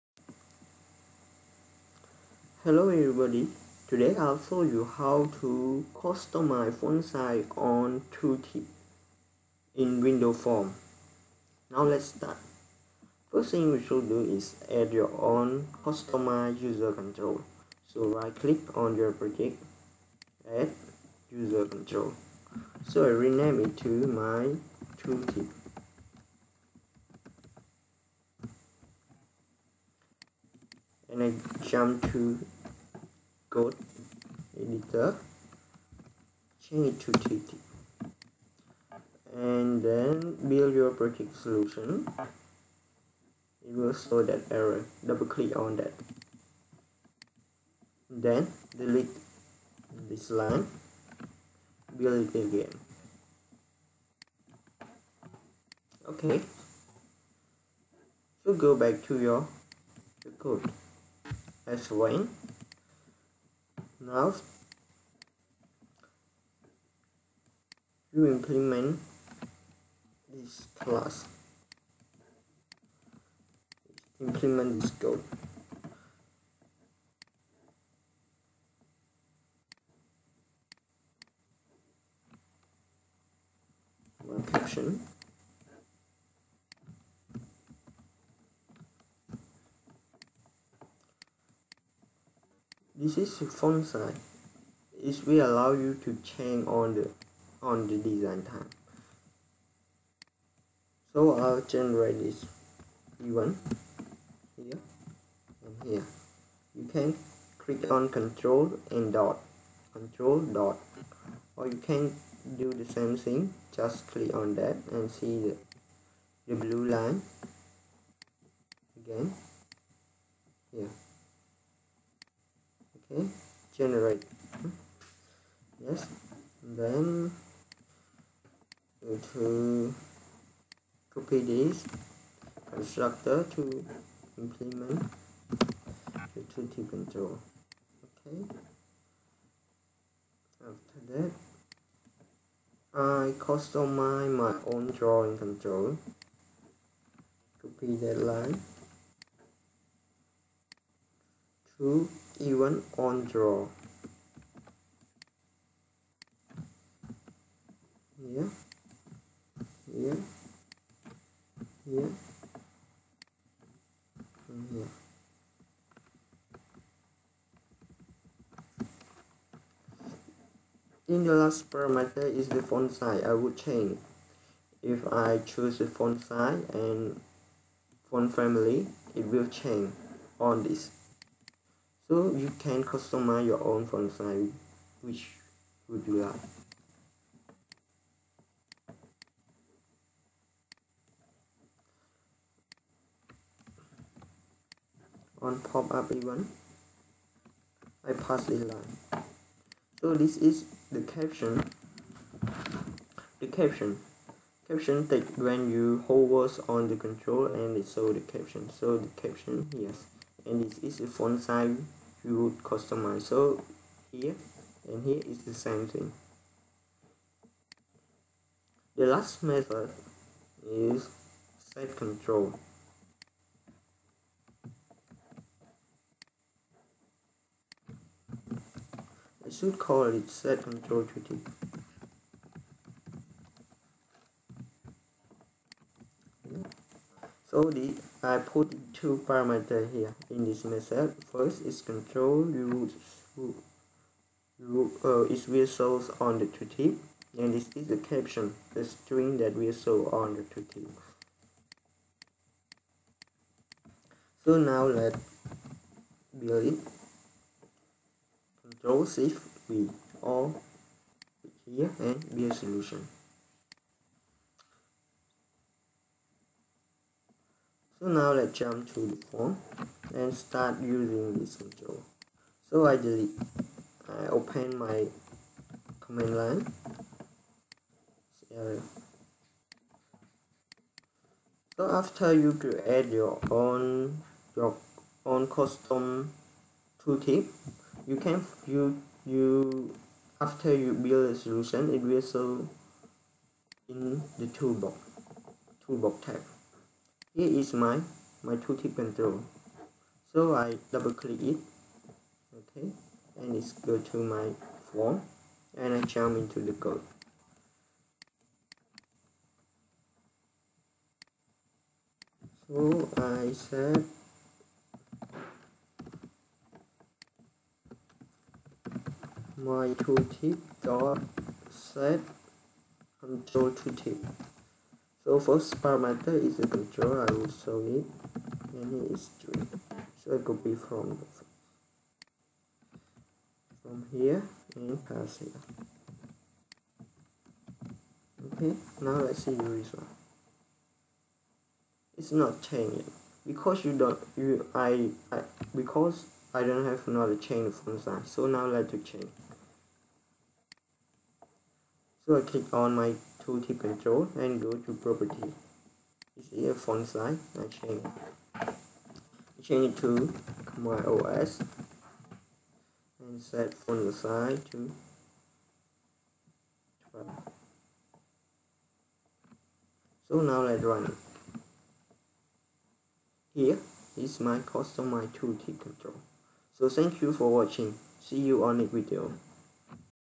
How to change tooltip font size control in window form
this viedo will show you how to change your tooltip font size
//Should Change event OnPopup in your custom ToolTip control for dynamic tooltip size. Thank :)
private void OnPopup(object sender, PopupEventArgs e)
      e.ToolTipSize = TextRenderer.MeasureText(this.GetToolTip(e.AssociatedControl), new Font("Khmer OS Content", 10F));